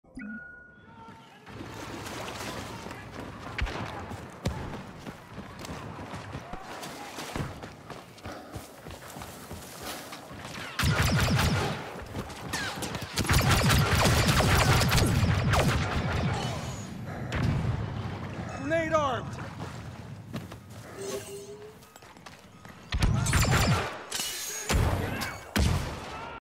2nd time i get a random grenade throw in a beta! (first was bf1)
Make sure to share the videos if you like em to twitter or facebook or google+ or any other place you can share it too!

PlayStation ID : ANIMALHOUSE55555